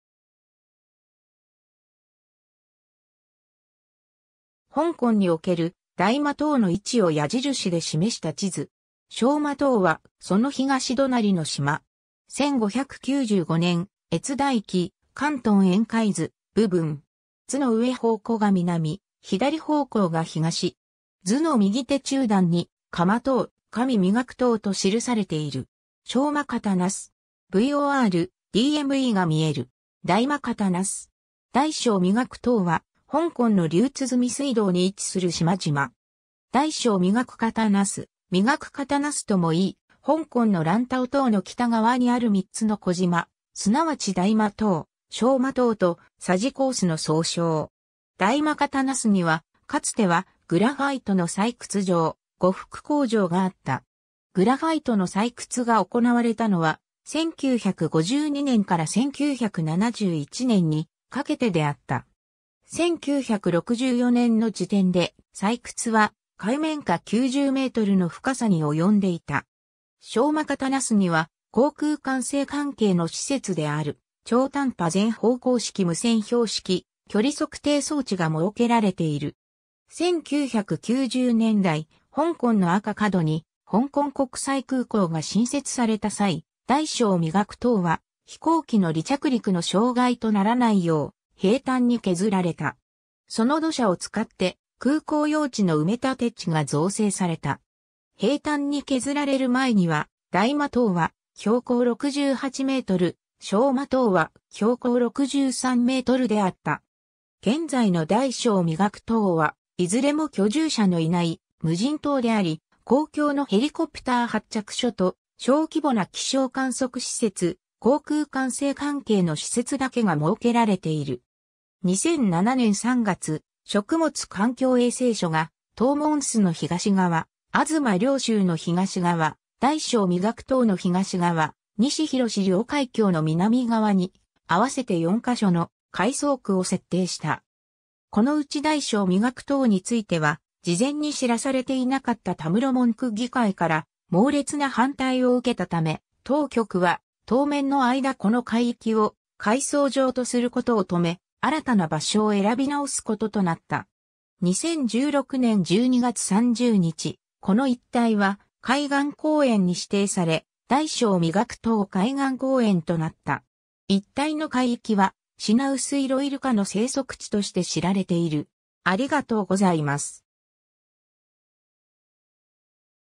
大小磨刀
香港における大磨刀の位置を矢印で示した地図。
小磨刀は、その東隣の島。
 1595年（明朝萬暦二十三年）『粤大記』「広東沿海図」部分。
図の上方向が南、左方向が東。
図の右手中断に、「下磨刀」、「上磨刀」と（右から左へ）記されている。
 小磨刀洲。
VOR/DMEが見える。
 大磨刀洲。
 大小磨刀（だいしょうまとう、英語: The Brothers、Tai Siu Mo To、Da Siu Mo To）は、香港の龍鼓水道に位置する島々。
大小磨刀洲（だいしょうまとうす、Tai Siu Mo To Chau）、磨刀洲（まとうす、Mo To Chau）ともいい、香港のランタオ島（大嶼山）の北側にある3つの小島、すなわち大磨刀（だいまとう、英語: West Brother、Tai Mo To）、小磨刀（しょうまとう、英語: East Brother、Siu Mo To）と、匙羹洲（しかんす、英語: Tsz Kan Chau）の総称。
 大磨刀洲には、かつてはグラファイト（石墨）の採掘場「五福礦場」があった。
グラファイトの採掘が行われたのは、1952年から1971年にかけてであった。
1964年の時点で、採掘は海面下90mの深さに及んでいた。
 小磨刀洲には、航空管制関係の施設である超短波全方向式無線標識 (VOR)/距離測定装置 (DME) が設けられている。
 1990年代、香港の赤鱲角に香港国際空港が新設された際、大小磨刀は飛行機の離着陸の障害とならないよう、平坦に削られた。
その土砂を使って空港用地の埋立地が造成された。
平坦に削られる前には、大磨刀は標高68m、小磨刀は標高63mであった。
現在の大小磨刀は、いずれも居住者のいない無人島であり、公共のヘリコプター発着所と、小規模な気象観測施設、航空管制関係の施設だけが設けられている。
 2007年3月、食物環境衛生署が、塔門洲の東側、東龍洲の東側、大小磨刀の東側、西博寮海峡の南側に、合わせて4か所の「海葬区」（海洋散骨のための海域）を設定した。
このうち大小磨刀については、事前に知らされていなかった屯門区議会から猛烈な反対を受けたため、当局は、当面の間この海域を海葬場とすることを止め、新たな場所を選び直すこととなった。
 2016年12月30日、この一帯は、海岸公園に指定され、大小磨刀海岸公園となった。
一帯の海域は、シナウスイロイルカ（中華白海豚）の生息地として知られている。